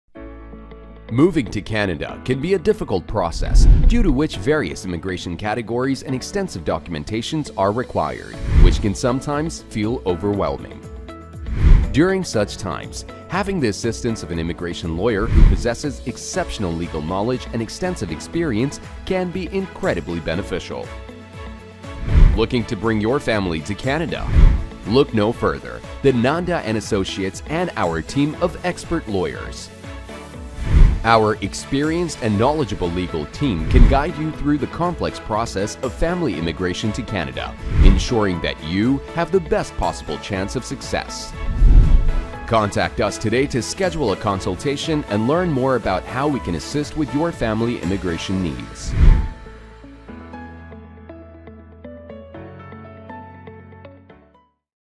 Family Immigration to Canada Made Easy with Nanda Associates
Moving to Canada can be a daunting task with complex immigration categories and extensive documentation requirements.

In such situations, it is crucial to have the assistance of an experienced immigration lawyer.

At Nanda Associates, we have a team of expert lawyers who can help you navigate the family immigration process with ease.

Whether you're looking to sponsor a spouse, partner, child, or any other family member, we have the expertise to guide you through the process and ensure that you have the best possible chance of success.